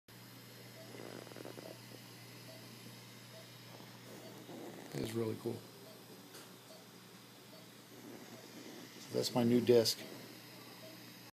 My new L5S1 disk or How I became a cyborg
Here's a quick look at the device that has replaced the L5S1 disk in my back via the Anterior Lumbar Discectomy and Fusion performed October 5th.